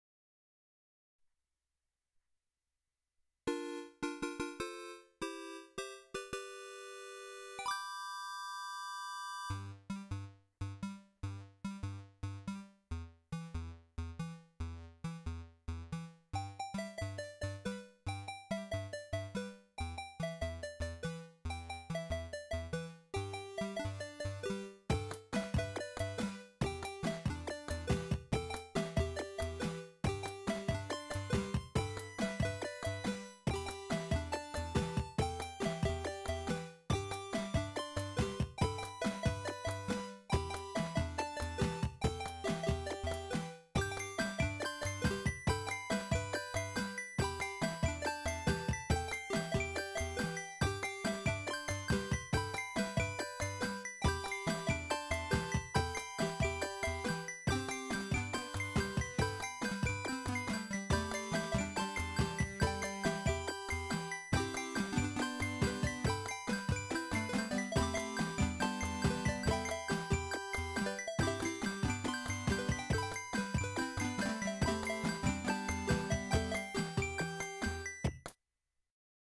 DANIEL DOES DISHES (MY VIDEO GAME SONG)
I made some vintage video game music and didn't know what kind of video to make with it and I needed to do the dishes.